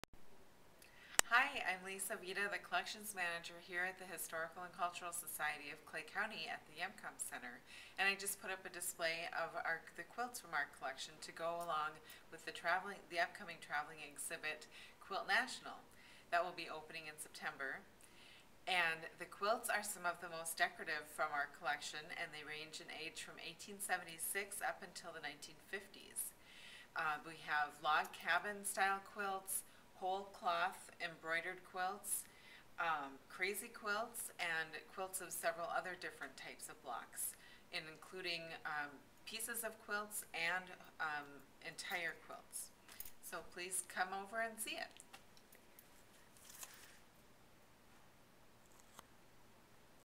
Clay County quilts and other artifcats
Here's a preview of a new hall case exhibit, curated by HCSCC Collections Manager Lisa Vedaa. The exhibit complements the upcoming Quilt National '09 exhibit and includes Clay County quilts and other artifacts.